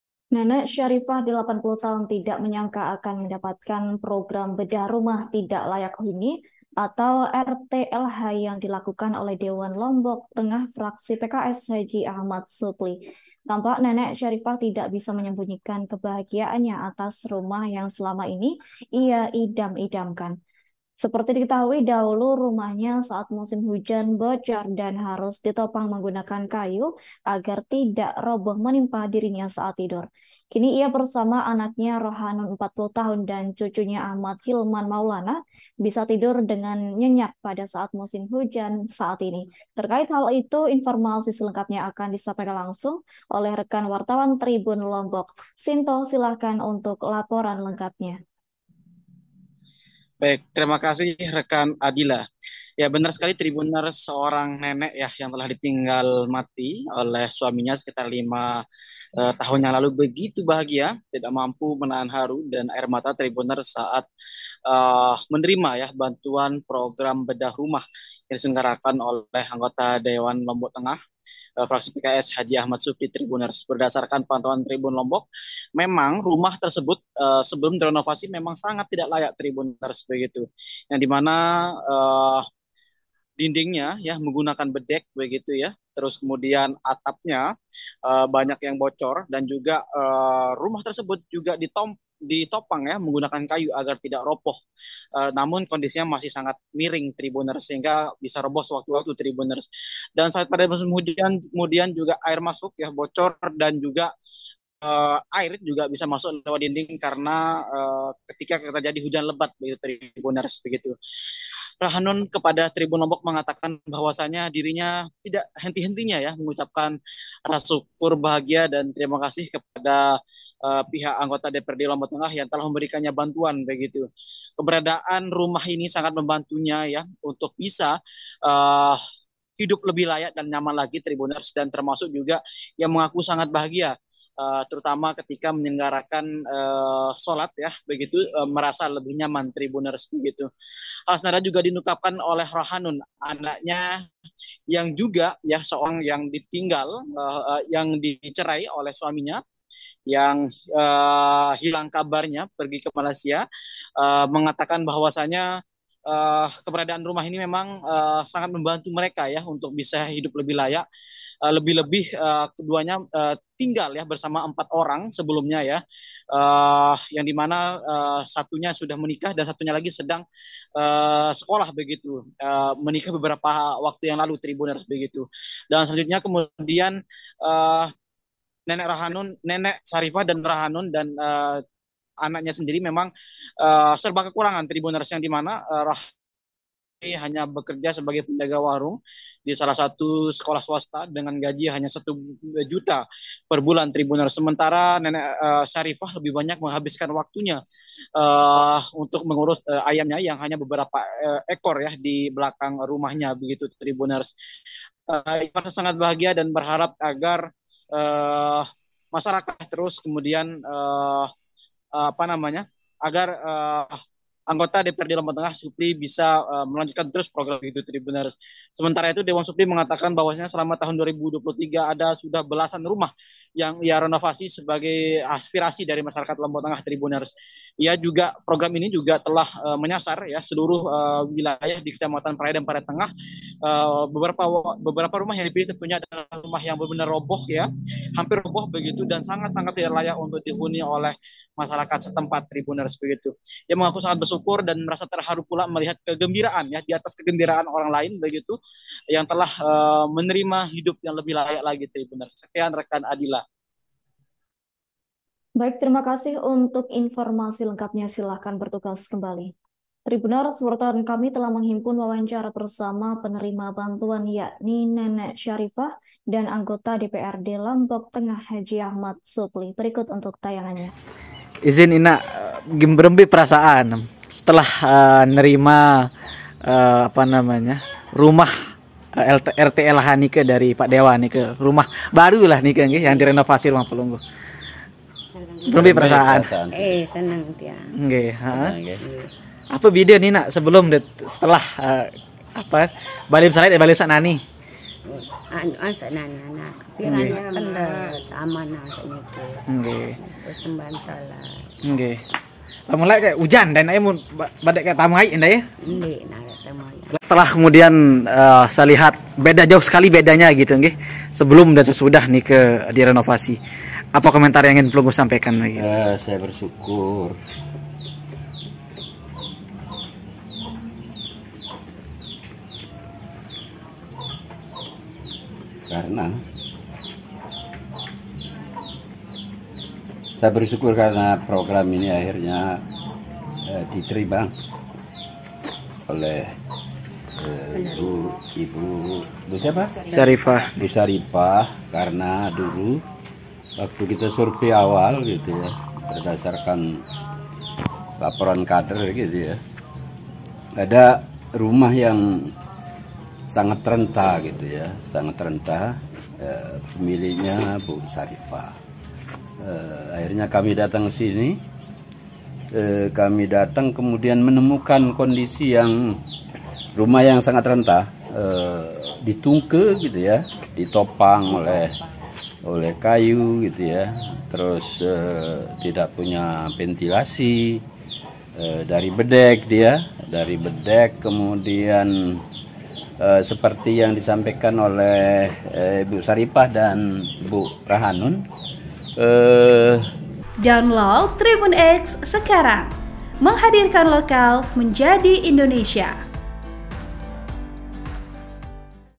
Kisah Nenek 80 Tak Menyangka Dapat Program Bedah Rumah: Dahulu Rumah Bocor, Kini Bisa Tidur Nyenyak
Laporan wartawan Tribun Lombok-Sinto

TRIBUN-VIDEO.COM - Nenek Syarifah (80) tidak menyangka bakal mendapatkan program bedah rumah tidak layak huni (RTLH) yang dilakukan oleh dewan Lombok Tengah fraksi PKS H Ahmad Supli.

Tampak nenek Syarifah tidak bisa menyembunyikan kebahagiaannya atas rumah yang selama ini ia idam-idamkan.

Dahulu rumahnya saat musim hujan bocor dan harus ditopang menggunakan kayu agar tidak roboh menimpa dirinya saat tidur.

Kini ia bersama anaknya Rohanun (40) dan cucunya Ahmad Hilman Maulana bisa tidur dengan begitu nyenyak pada saat musim hujan seperti saat ini. (*)

VP: Latif